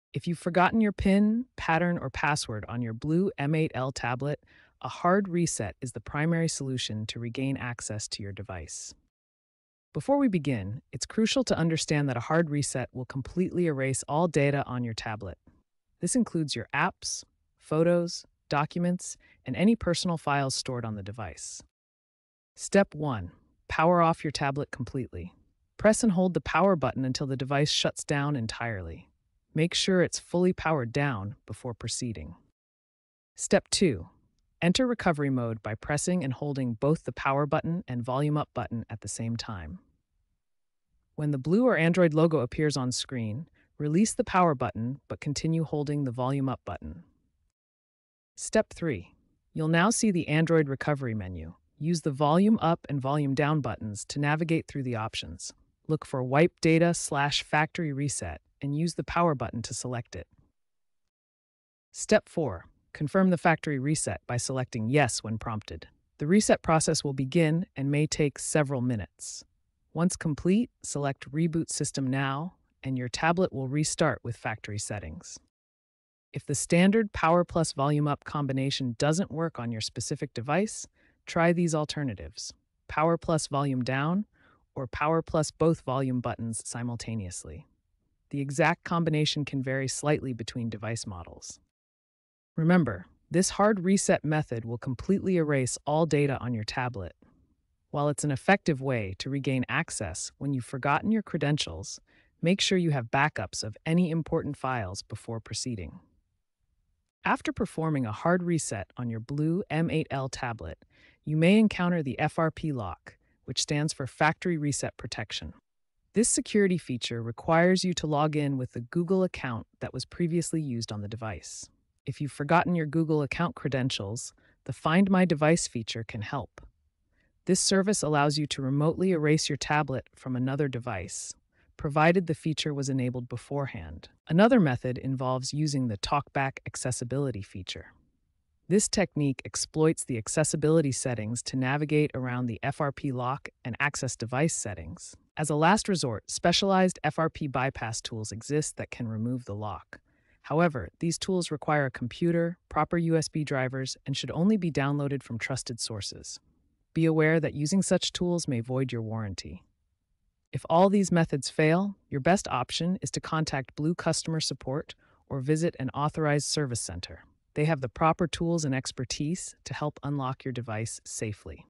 How To Unlock M8L Blu Tablet If Locked Or Forgot Pin, Pattern, Password Reset Guide
Are you struggling to access your M8L Blu Tablet due to a forgotten PIN, pattern, or password? This video provides a step-by-step reset guide to help you unlock your device with ease. We will explore different techniques, including safe mode and factory reset options, to ensure you can regain control of your tablet without hassle.